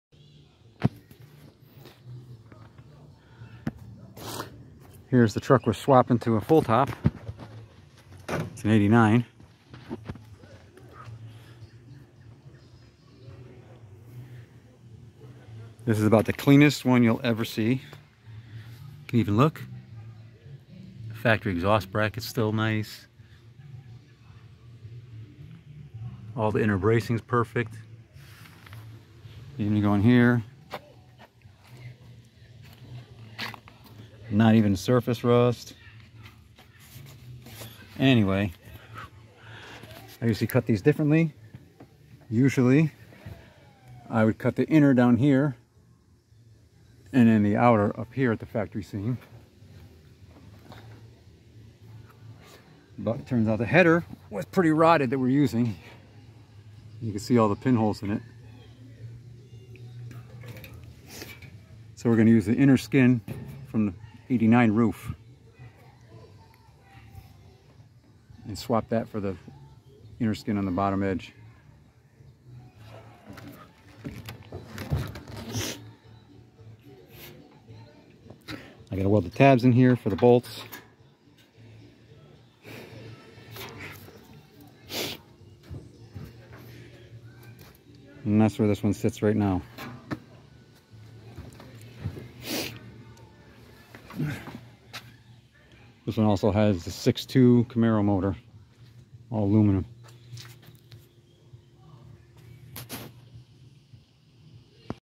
Blazer full top convertible conversion part 1.
Converting an 1989 Blazer to a full top convertible.